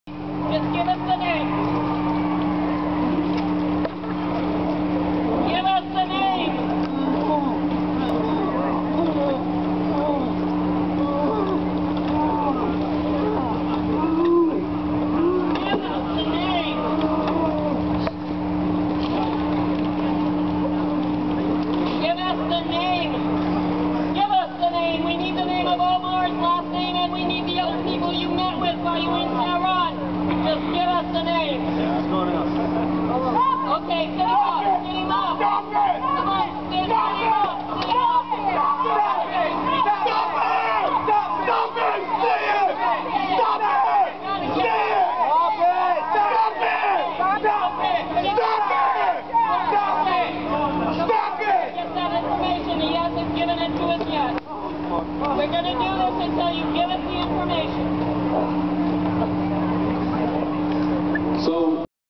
Anti-Torture Protest at UC Berkeley مظاهرة ضد التعذيب في جامعة كاليفورنيا بيركلي
Activists simulating waterboarding at UC Berkeley